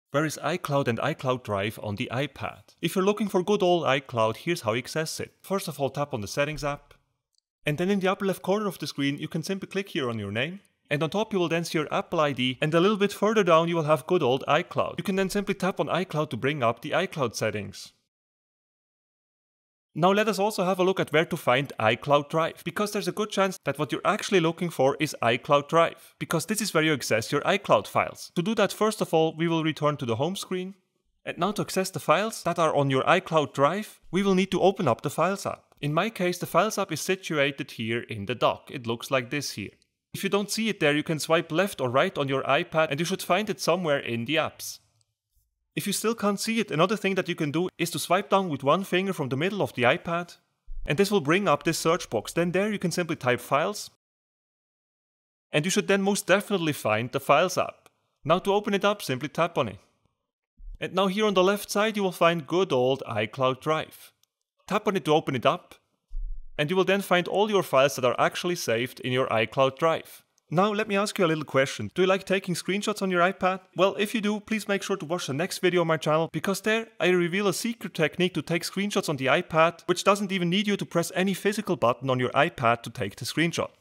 Where is iCloud and iCloud Drive on the iPad — SOLVED
In this video, we'll help you figure out where iCloud and iCloud Drive are on your iPad. We'll show you how to access these files and folders, and explain what they're used for.

If you're having trouble finding iCloud or iCloud Drive on your iPad, then this video may be helpful. We'll show you where these files and folders are on your iPad and explain what they're used for. After watching this video, you'll know how to access these files and folders on your iPad!